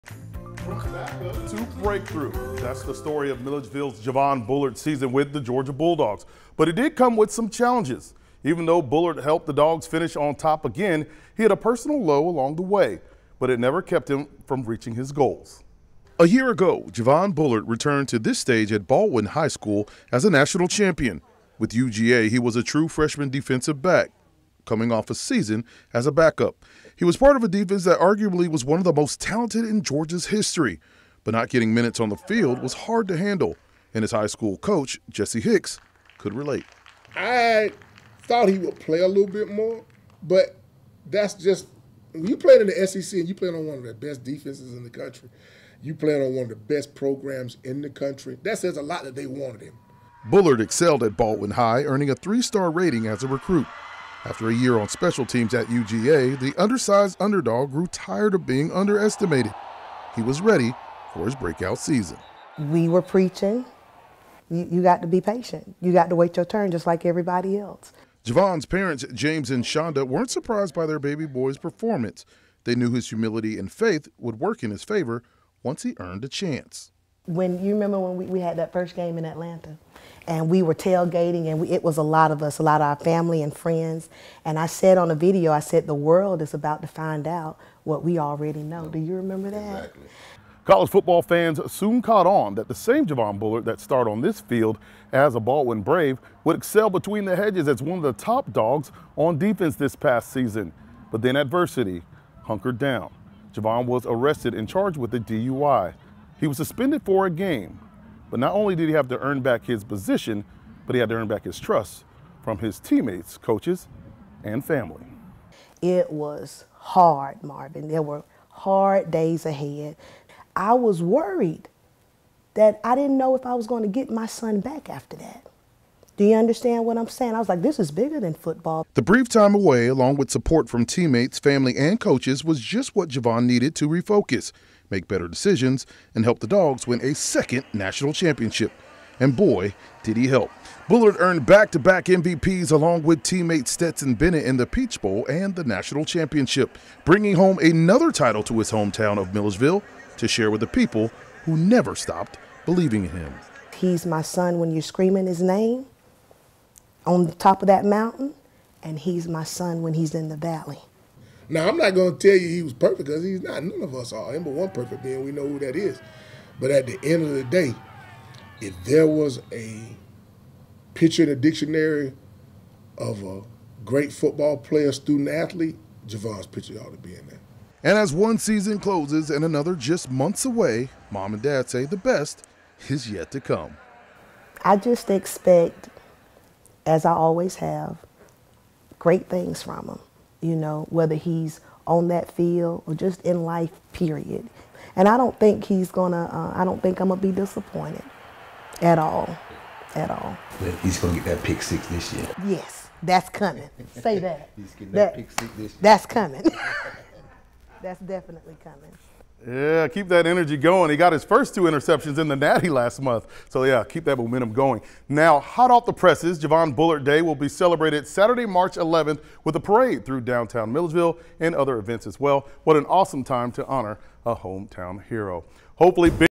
From backup to breakthrough: Milledgeville's Javon Bullard journey to stardom with Georgia Bulldogs
Bullard alongside Stetson Bennett took home a pair of MVP awards in the Peach Bowl and the national championship.